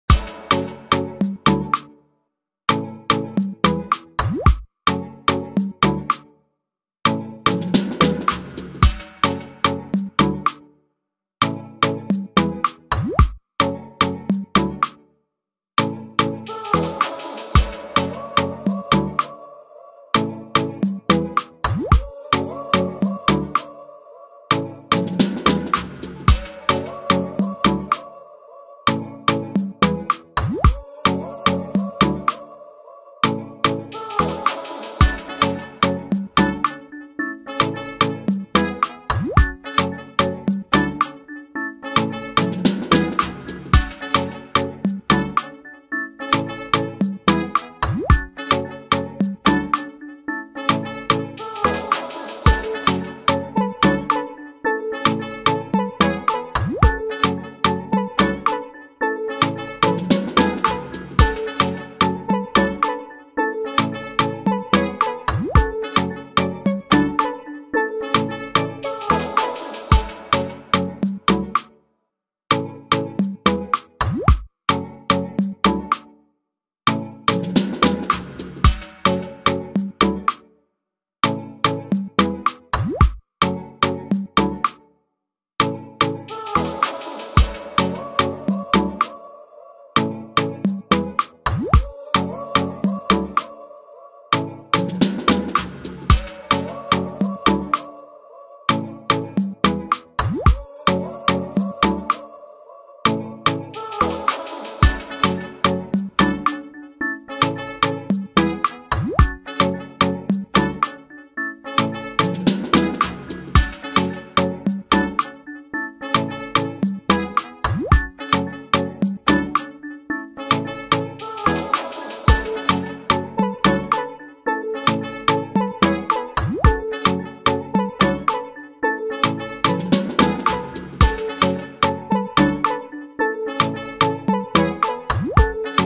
Comment réparer Gold Note qui ne s'affiche pas sur Instagram 2024 | Gold Note ne fonctionne pas
Comment réparer Gold Note qui ne s'affiche pas sur Instagram 2024 | Gold Note ne fonctionne pas sur Instagram

Si votre Gold Note n'apparaît pas sur Instagram, vous pouvez essayer quelques solutions pour y remédier. Assurez-vous d'utiliser la dernière version de l'application. Vérifiez l'App Store ou Google Play pour connaître les mises à jour. Parfois, vider le cache peut aider à résoudre les problèmes. Accédez aux paramètres de votre appareil, recherchez Instagram et videz le cache. Désinstallez et réinstallez l'application pour vous assurer qu'elle fonctionne correctement. Assurez-vous d'avoir une connexion Internet stable. Une connexion faible peut empêcher les fonctionnalités de se charger correctement.

Si aucune de ces solutions ne fonctionne, il peut s'agir d'un problème temporaire du côté d'Instagram. Attendez un peu et réessayez plus tard.